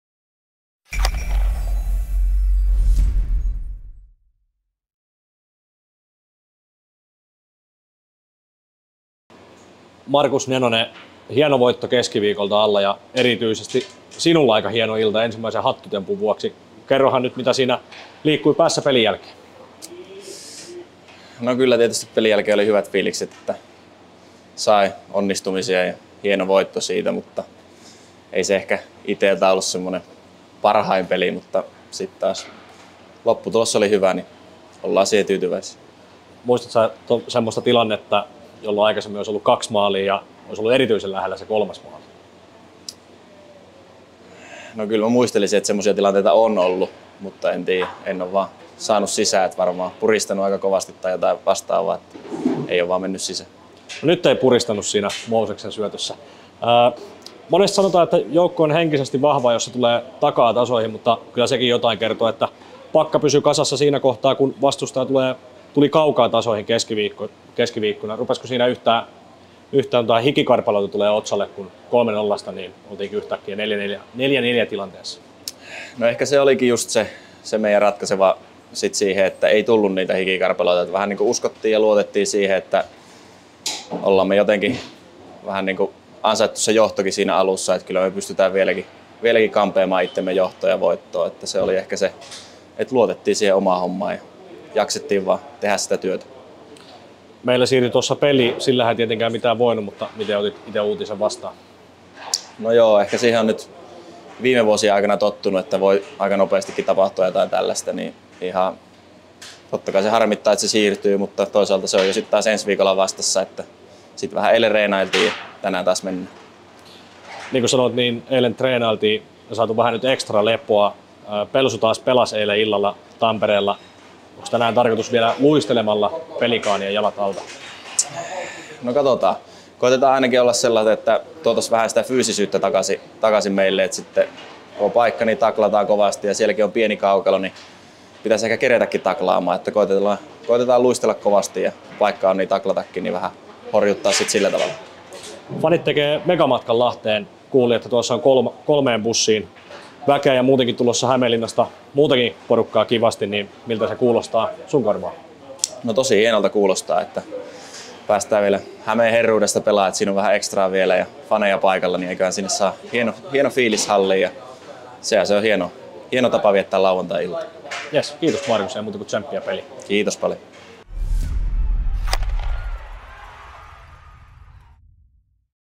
BioRex-otteluennakko Pelicans-HPK 26.11.2022 - haastattelussa Markus Nenonen
Markus Nenonen kertaa edellisen ottelun hattutemppuaan ja ennakoi illan matsia Pelicans-HPK.